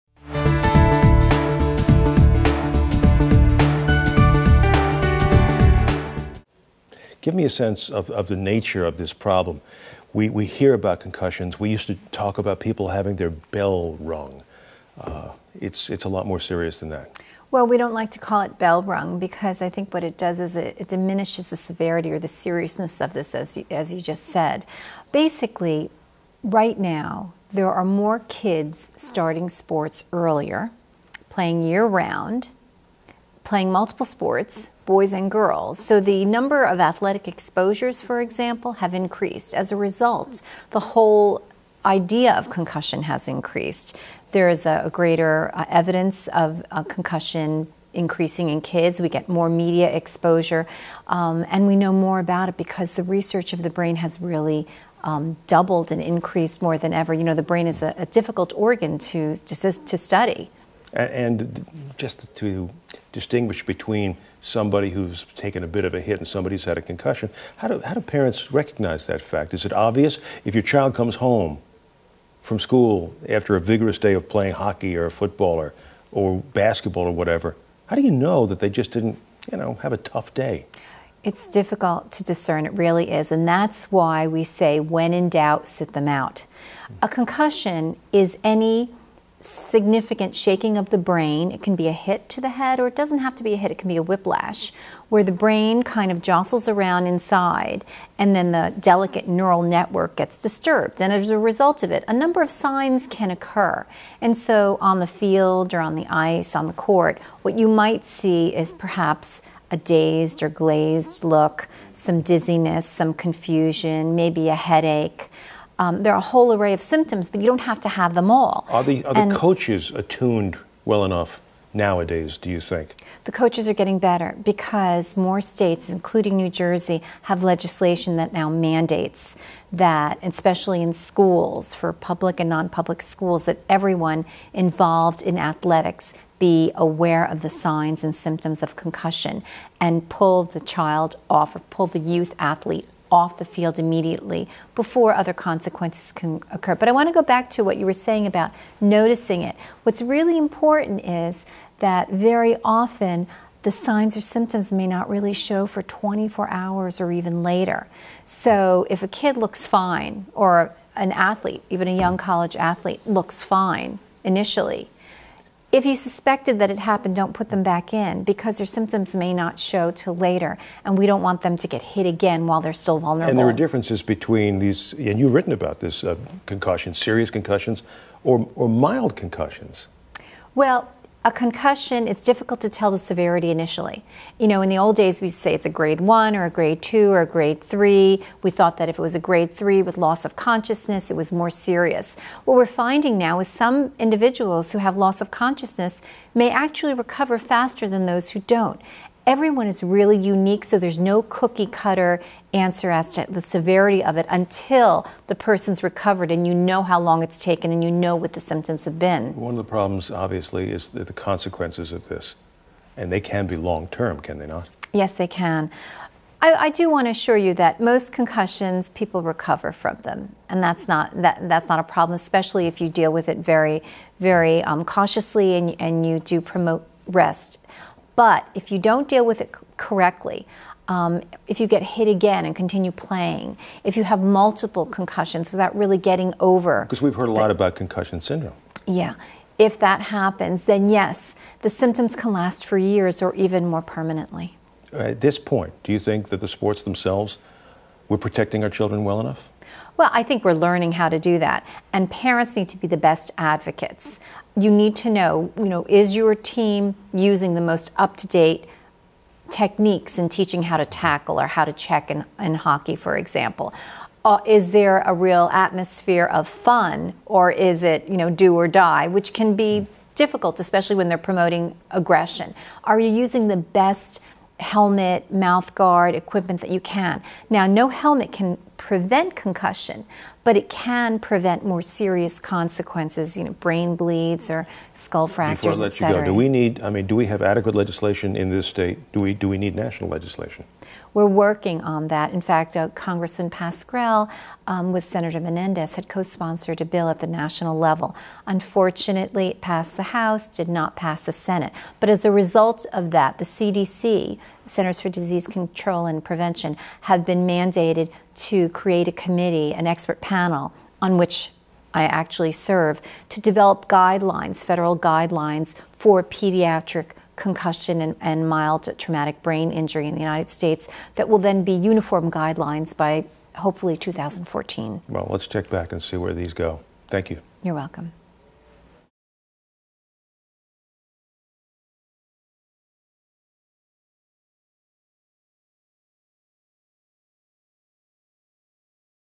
Rate of Concussions in Kids Increasing, Symptoms Not Immediately Apparent
Concussions are serious injuries that have been discussed more in depth recently as doctors have learned more about the head injuries. Sports Concussion Center of New Jersey Director Rosemarie Moser told NJ Today Managing Editor Mike Schneider that rest is important in the recovery process. She also said parents need to be vigilant to protect their children from recurring sports injuries. For more New Jersey news, visit NJ Today